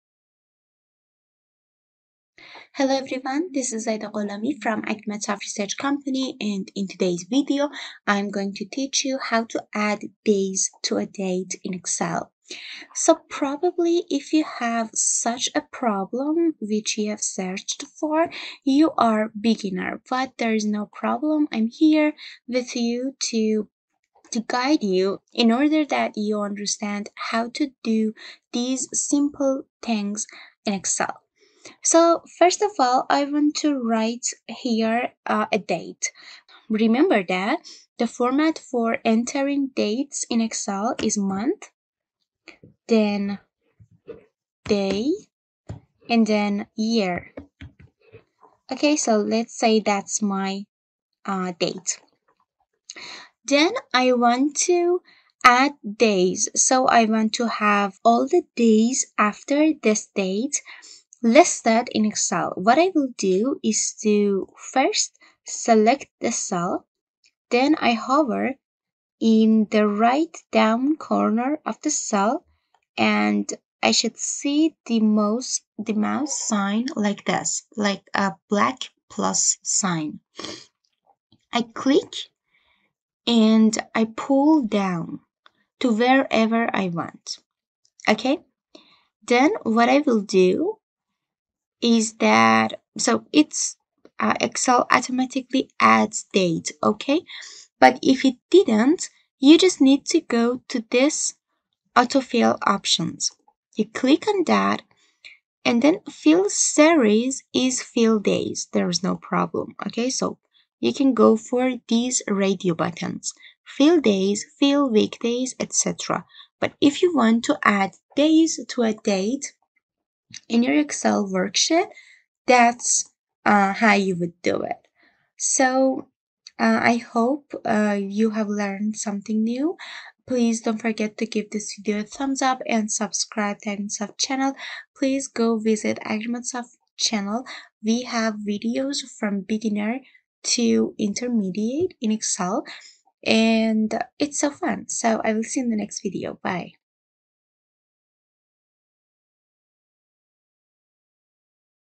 Add Days to a Date in Excel
This is one of the easiest tutorials for excel! There are two ways. One is through the autofill option and the other is to add one to the first day and copy the formula up to the desired date. If you have such problems, probably you are new to excel which is very cool! I recommend you to watch other videos on the Agrimetsoft channel.